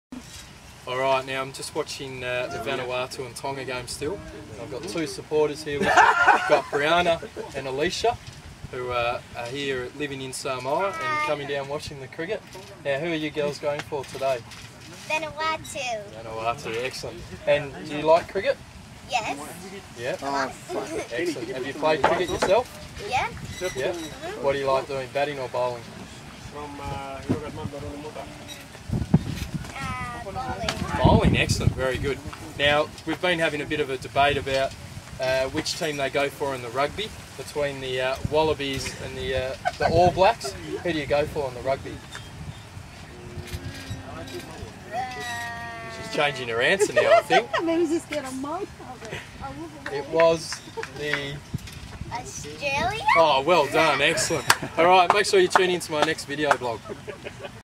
Charlie Burke with Vanuatu Supporters at 2009 EAP Trophy.flv
Charlie Burke interviews two young cricket fans at the Vanuatu v Tonga game in his ICC EAP Trophy blog